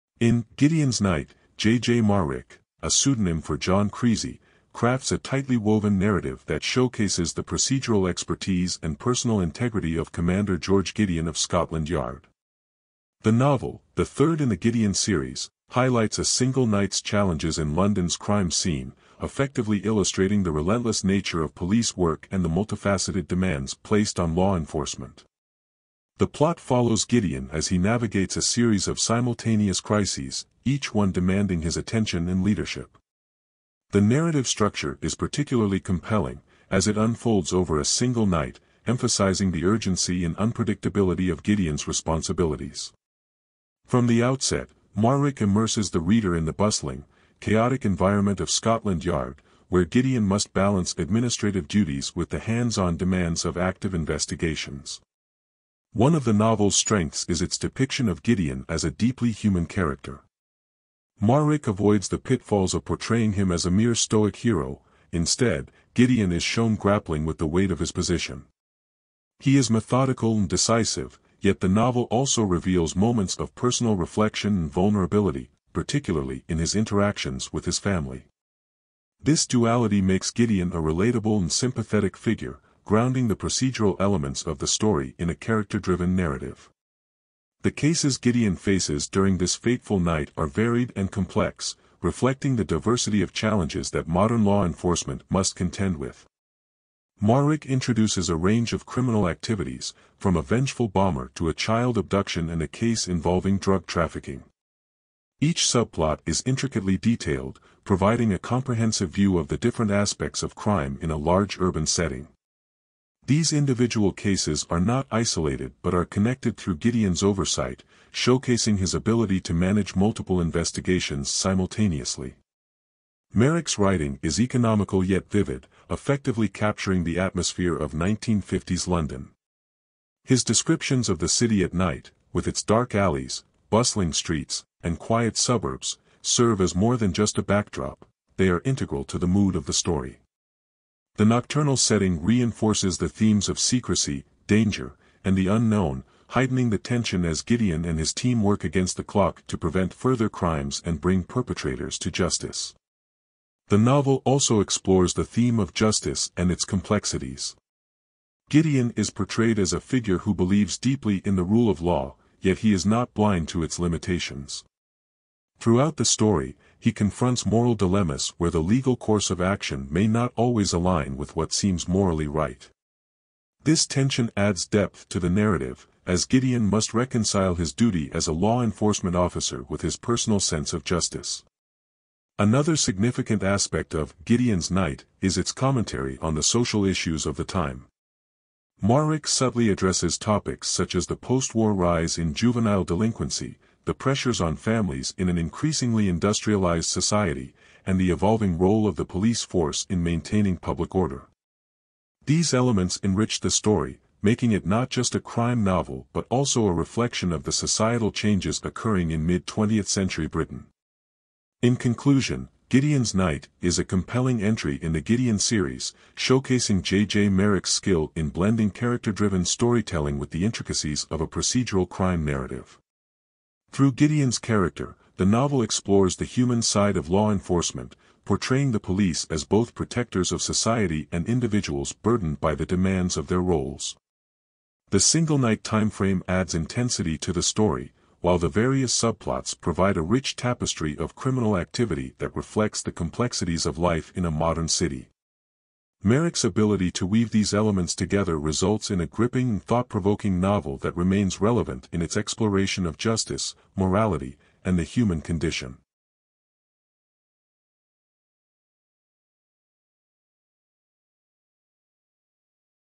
"Gideon's Night (Gideon, #3)" By J.J. Marric
In "Gideon's Night," J.J. Marric (a pseudonym for John Creasey) crafts a tightly woven narrative that showcases the procedural expertise and personal integrity of Commander George Gideon of Scotland Yard. The novel, the third in the Gideon series, highlights a single night's challenges in London's crime scene, effectively illustrating the relentless nature of police work and the multifaceted demands placed on law enforcement.The plot follows Gideon as he navigates a series of simultaneous crises, each one demanding his attention and leadership. The narrative structure is particularly compelling, as it unfolds over a single night, emphasizing the urgency and unpredictability of Gideon’s responsibilities. From the outset, Marric immerses the reader in the bustling, chaotic environment of Scotland Yard, where Gideon must balance administrative duties with the hands-on demands of active investigations.One of the novel's strengths is its depiction of Gideon as a deeply human character. Marric avoids the pitfalls of portraying him as a mere stoic hero; instead, Gideon is shown grappling with the weight of his position. He is methodical and decisive, yet the novel also reveals moments of personal reflection and vulnerability, particularly in his interactions with his family. This duality makes Gideon a relatable and sympathetic figure, grounding the procedural elements of the story in a character-driven narrative.The cases Gideon faces during this fateful night are varied and complex, reflecting the diversity of challenges that modern law enforcement must contend with. Marric introduces a range of criminal activities, from a vengeful bomber to a child abduction and a case involving drug trafficking. Each subplot is intricately detailed, providing a comprehensive view of the different aspects of crime in a large urban setting. These individual cases are not isolated but are connected through Gideon’s oversight, showcasing his ability to manage multiple investigations simultaneously.Marric’s writing is economical yet vivid, effectively capturing the atmosphere of 1950s London. His descriptions of the city at night, with its dark alleys, bustling streets, and quiet suburbs, serve as more than just a backdrop—they are integral to the mood of the story. The nocturnal setting reinforces the themes of secrecy, danger, and the unknown, heightening the tension as Gideon and his team work against the clock to prevent further crimes and bring perpetrators to justice.The novel also explores the theme of justice and its complexities. Gideon is portrayed as a figure who believes deeply in the rule of law, yet he is not blind to its limitations. Throughout the story, he confronts moral dilemmas where the legal course of action may not always align with what seems morally right. This tension adds depth to the narrative, as Gideon must reconcile his duty as a law enforcement officer with his personal sense of justice.Another significant aspect of "Gideon's Night" is its commentary on the social issues of the time. Marric subtly addresses topics such as the post-war rise in juvenile delinquency, the pressures on families in an increasingly industrialized society, and the evolving role of the police force in maintaining public order. These elements enrich the story, making it not just a crime novel but also a reflection of the societal changes occurring in mid-20th century Britain.In conclusion, "Gideon's Night" is a compelling entry in the Gideon series, showcasing J.J. Marric's skill in blending character-driven storytelling with the intricacies of a procedural crime narrative. Through Gideon's character, the novel explores the human side of law enforcement, portraying the police as both protectors of society and individuals burdened by the demands of their roles. The single-night timeframe adds intensity to the story, while the various subplots provide a rich tapestry of criminal activity that reflects the complexities of life in a modern city. Marric's ability to weave these elements together results in a gripping and thought-provoking novel that remains relevant in its exploration of justice, morality, and the human condition.